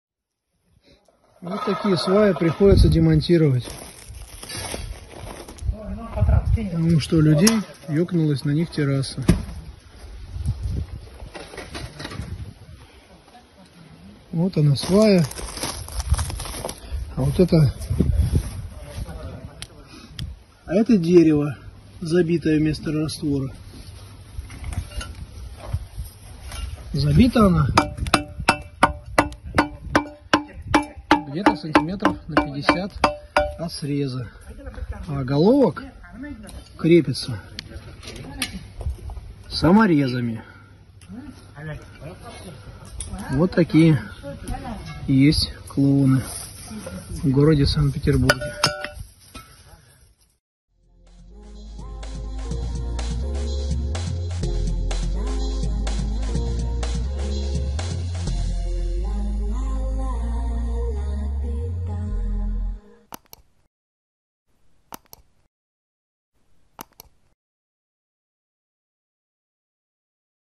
ВИНТОВОЙ ФУНДАМЕНТ ИЛИ СТРОИТЕЛЬНЫЙ КОСЯК ПО ПИТЕРСКИ
Винтовой фундамент из говна и палок - немного постояла и упала терраса.
Это ещё одна печальная история заказчика, которому пришлось демонтировать строение, чтобы заменить весь фундамент.

Изначально, человек отказался от наших услуг потому, что было дорого.
В последствие человек заплатил в 5 (пять) раз больше, чем, если бы он сделал сразу правильно!

Затраты на исправление проблемы состояли из следующих позиций:
- демонтаж террасы
- демонтаж аварийного винтового фундамента
- монтаж нового винтового фундамента
- сборка террасы

В данном случае всем известная поговорка про «скупого» нервно курит в сторонке…
Не удивительно, почему к винтовым сваям у многих людей сформировалось очень негативное отношение. Его создали клоуны, которые наводняют рынок строительного шоу-бизнеса.
Хотя на самом деле винтовые сваи, это отличная технология, позволяющая решать многие вопросы. Особенно на сложных грунтах и перепадах высот.
Ранее нам уже приходилось видеть дерево, которое заменяло монолитные пояса и перемычки в доме из газобетона. Это фактически аварийный дом. Но чтобы вместо цементно-песчаной смеси заколачивали дерево внутрь сваи, а потом вместо сварки оголовки прикручивали саморезами к забитому дереву…